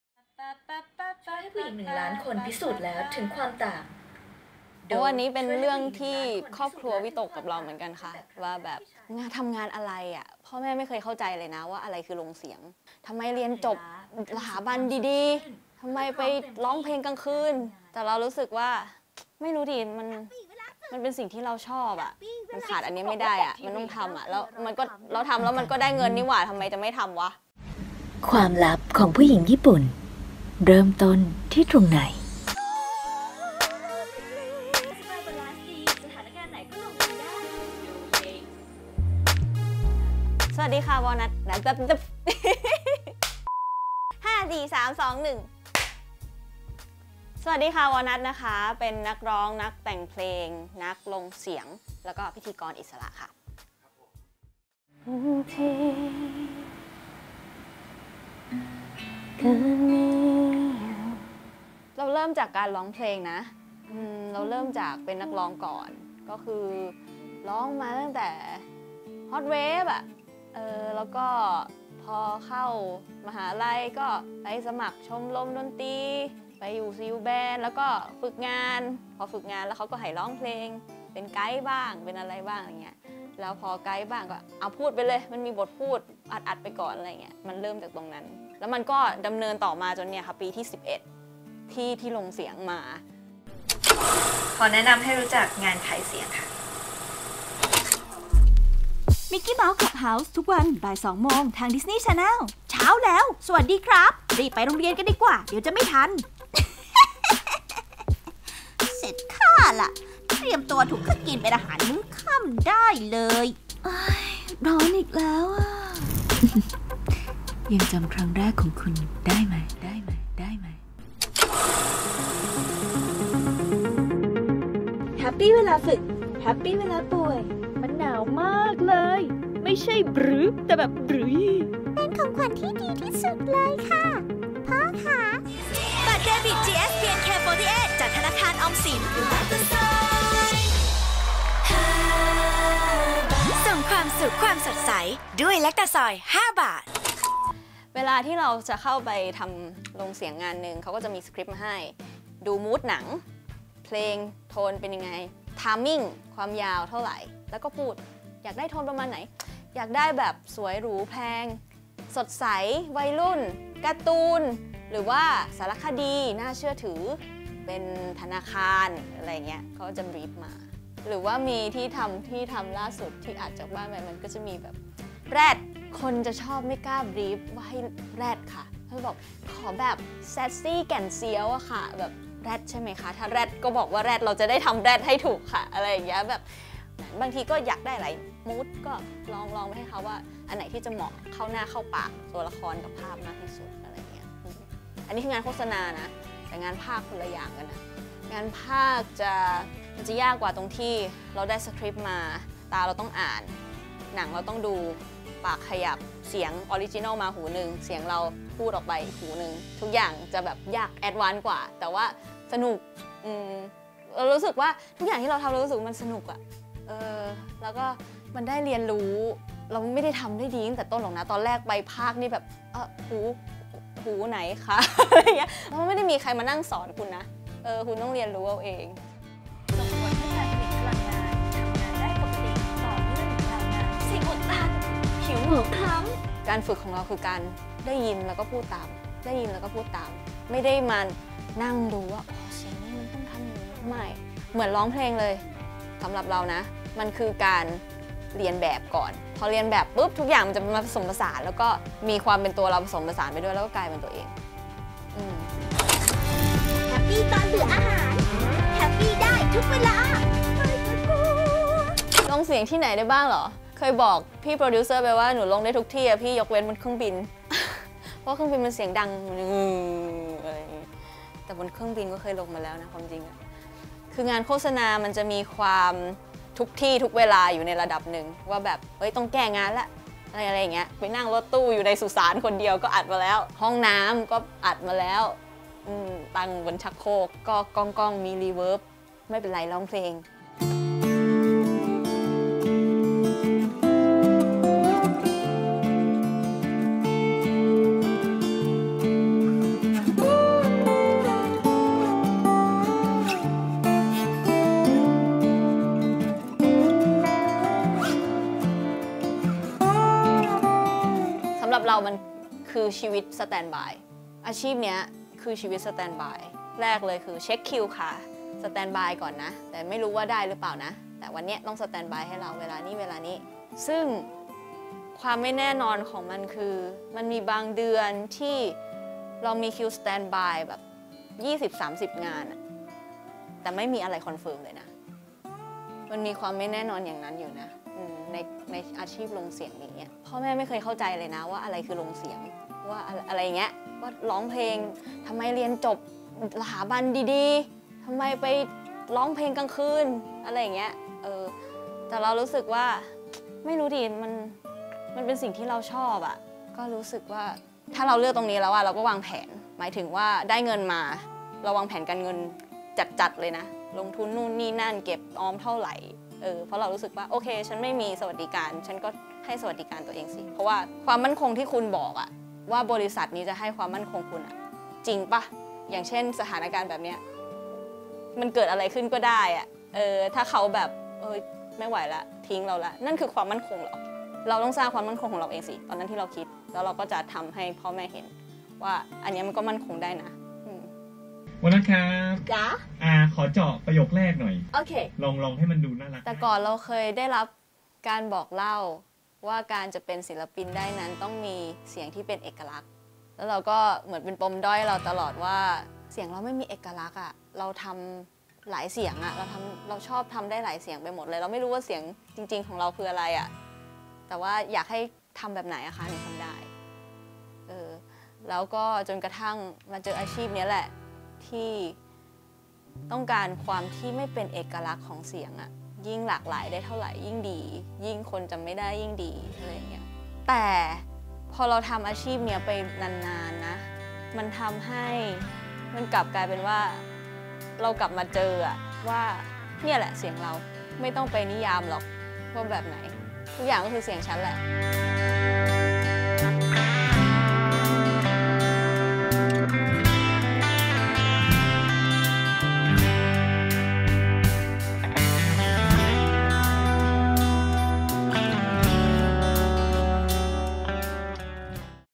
วอลนัท : คนขายเสียง ผู้อยู่เบื้องหลังเสียงโฆษณานับร้อยที่คุณไม่รู้ว่าใคร
"คืองานโฆษณามันจะมีความ ทุกที่ทุกเวลาอยู่ในระดับนึง ว่าแบบ เห้ย ต้องแก้งานละ นั่งรถตู้อยู่ในสุสานคนเดียวก็อัดมาแล้ว ห้องน้ำก็อัดมาแล้ว

พ่อแม่ไม่เคยเข้าใจเลยนะว่าอะไรคือลงเสียง ทำไมเรียนจบสถาบันดีๆ ทำไมไปร้องเพลงกลางคืน แต่เรารู้สึกว่า ไม่รู้ดิ มันเป็นสิ่งที่เราชอบอะ มันขาดอันนี้ไม่ได้อะ มันต้องทำอะ ทำแล้วมันก็ได้เงินนี่หว่า ทำไมจะไม่ทำวะ"
จากอาชีพที่พ่อแม่ไม่เคยเข้าใจ
จากอาชีพที่ทำเพราะใจรัก ไม่มีความมั่นคง
จากอาชีพที่ทำเป็นแค่งานอดิเรก
วันนี้ วอลนัท เธอเติมเต็มความฝันตัวเองด้วยการพิสูจน์ให้ทุกคนเห็นว่า เสียง ของเธอนั้นมีมูลค่าขนาดไหน เบื้องหลังเสียงโฆษณา, การ์ตูน หรือแม้แต่เสียงเพลงดังๆ ที่พวกเรารับชมและไม่รู้ว่าใครกัน เชื่อว่าถ้าพูดไล่เรียงออกไปทุกคนต้องร้องว่า "อ๋อ เสียงคนนี้หรอ"
ใช่แล้ว นี่คืออาชีพของเธอที่ทำมากว่า 11 ปี
จากอาชีพที่พ่อแม่ไม่เคยเข้าใจ เธอก็ทำให้พ่อแม่เข้าใจได้
จากอาชีพที่ทำเพราะใจรัก ไม่มีความมั่นคง เธอก็มอบสวัสดิการให้ตัวเองได้
จากอาชีพที่ทำเป็นแค่งานอดิเรก ตอนนี้มันกลายเป็นช่องทางหาเงินหลักๆ ไปแล้ว
เชิญชมเรื่องราวของเธอได้ในบทสัมภาษณ์นี้ "วอลนัท : คนขายเสียง"
**วอลนัทเคยเป็นนักร้องนำวง Apple Girls Band และประกวดร้องเพลงรายการ The Voice Thailand
เรื่อง : บุญญฤทธิ์ บัวขำ
ภาพ : พีระพงษ์ พงษ์นาค
ตัดต่อ : ปกป้อง เฉลิมพงศ์
กราฟฟิค : คุณากร หาญกล้า